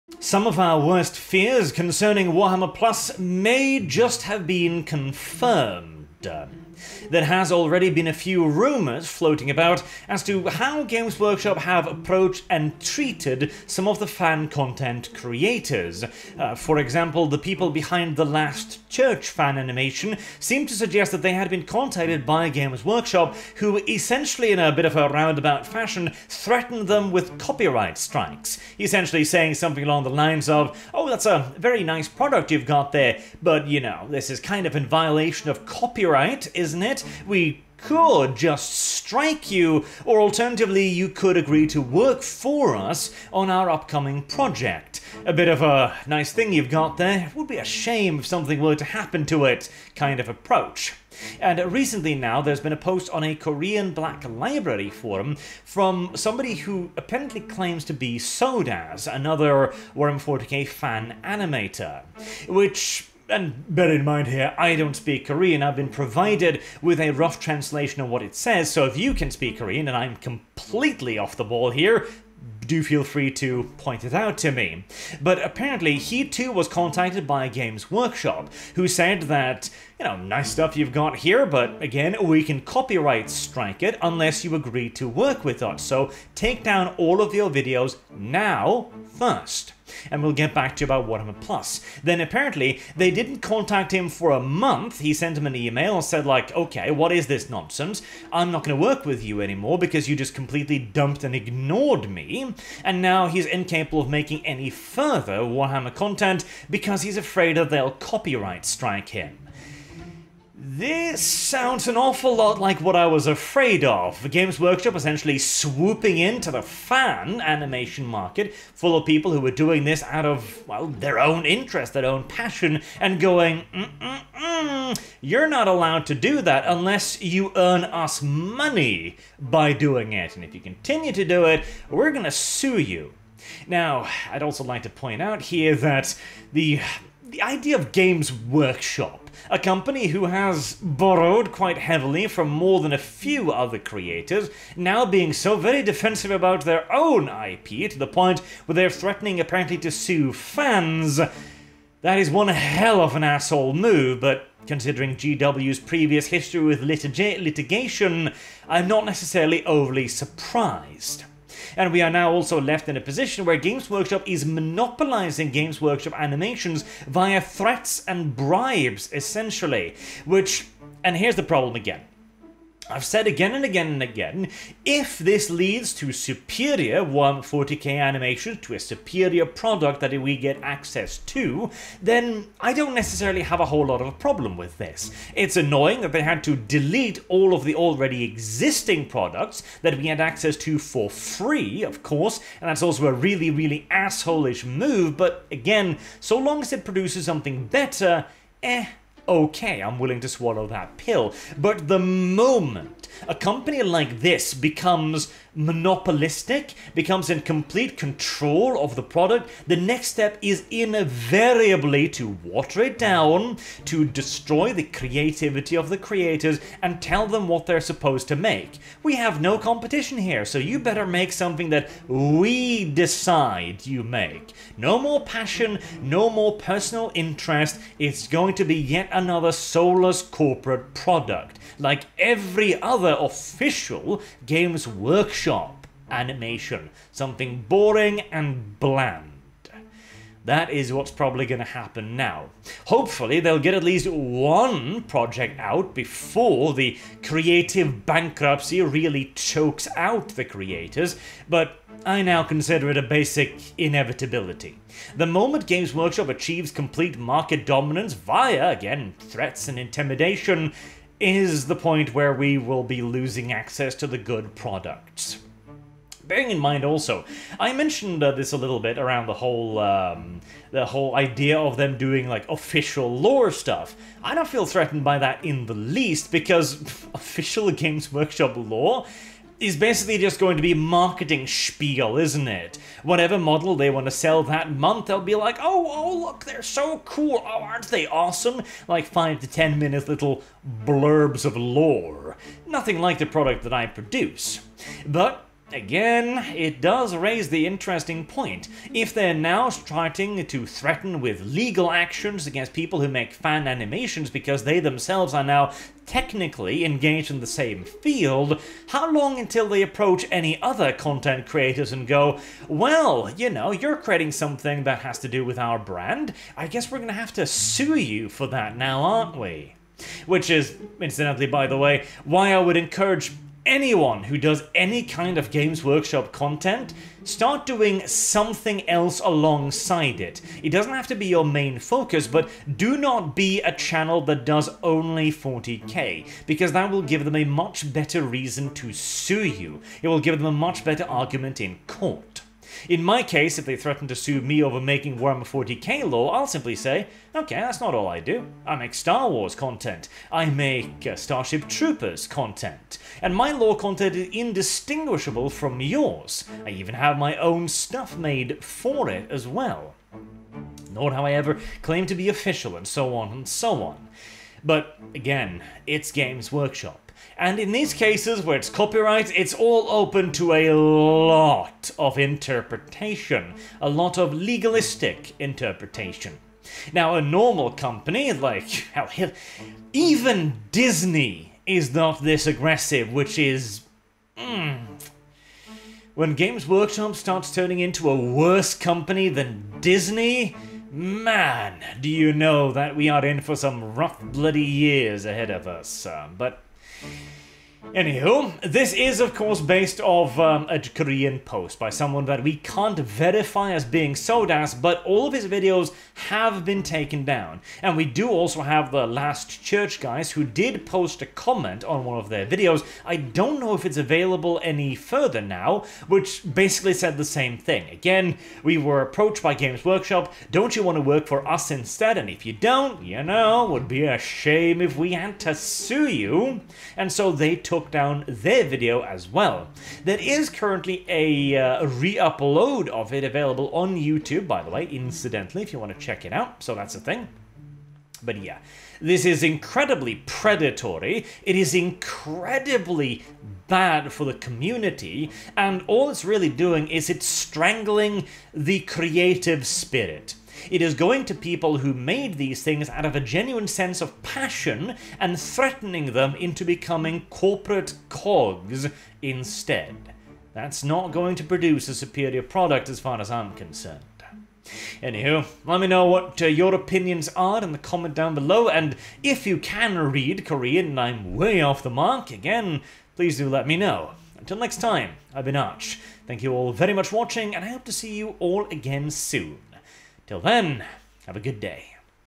Is GW Using Threat's To Force Creators To Delete Their Content? Then Ignore Them For Warhammer+?
Well, i would love to say i was surprised but any one with eyes to see could probably have seen this coming.